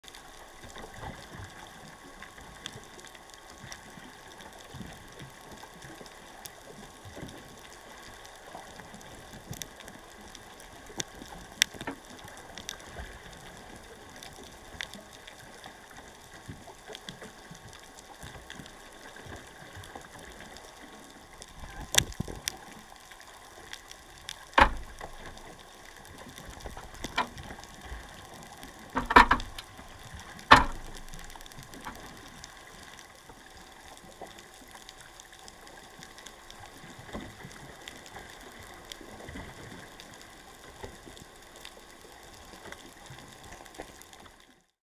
Fish Getting Caught In the Gulf of Mexico
Underwater footage of a red grouper, grunt, and porgy taking a jig in the Gulf of Mexico.